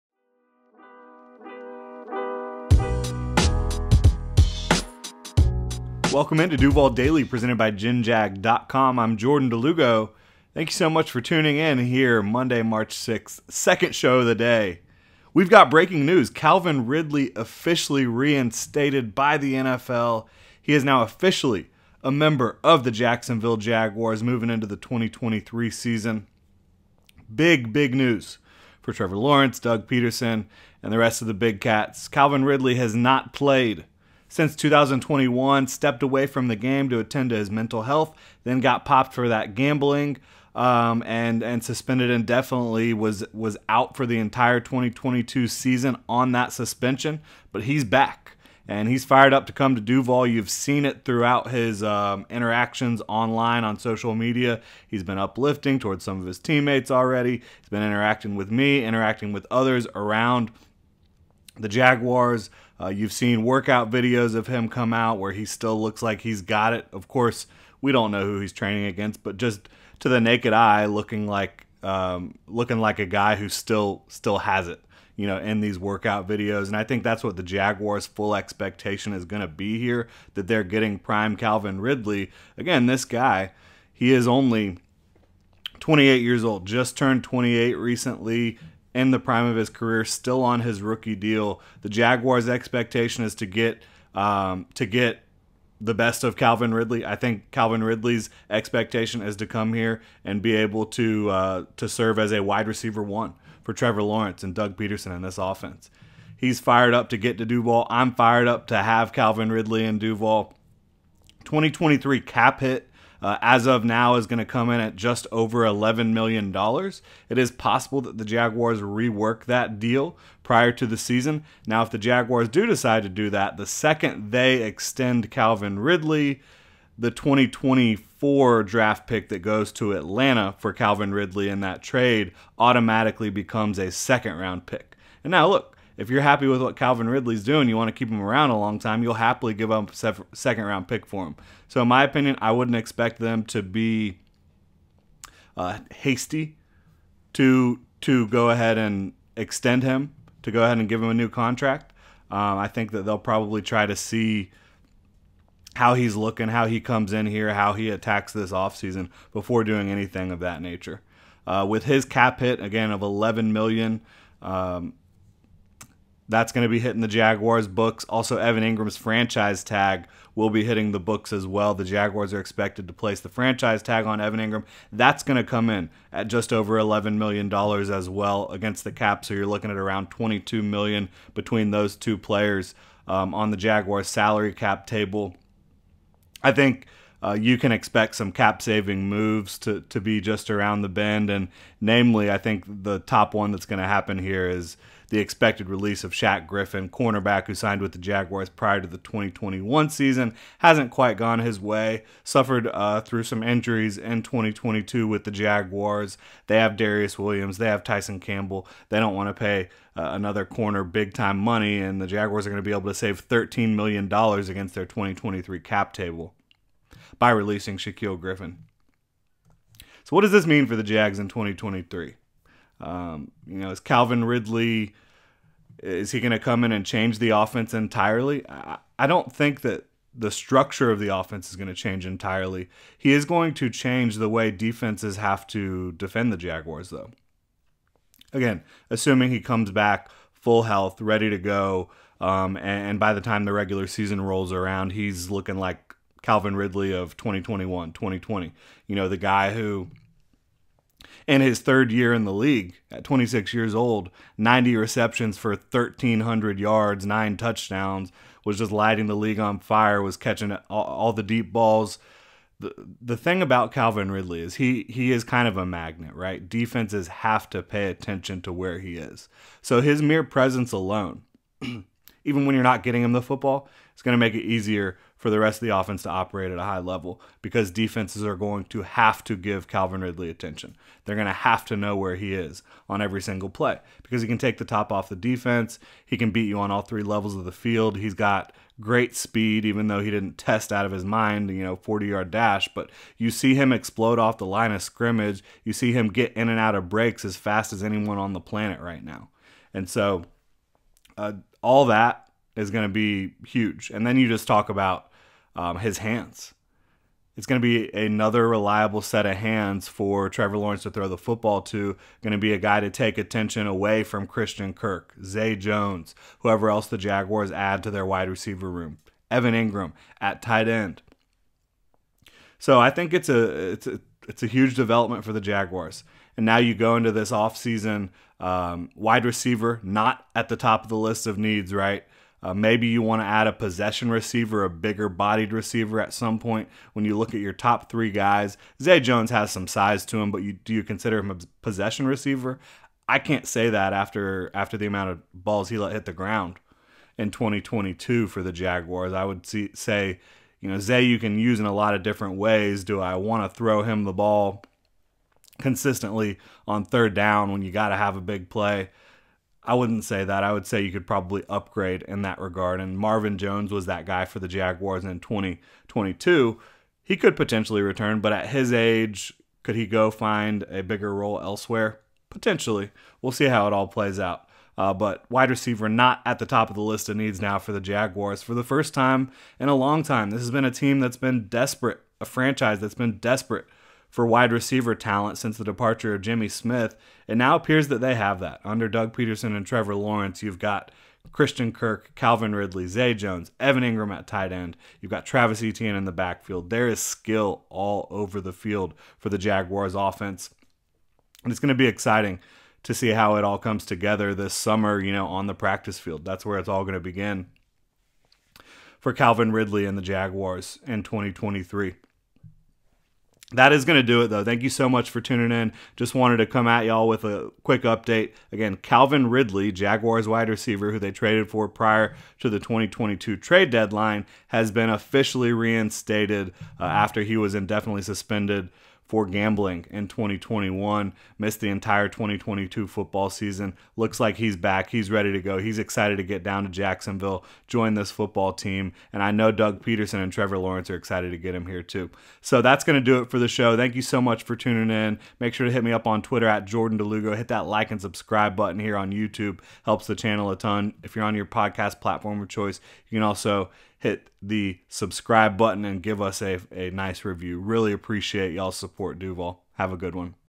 Calvin Ridley OFFICIALLY REINSTATED!
The NFL has officially reinstated Calvin Ridley to the Jaguars active roster.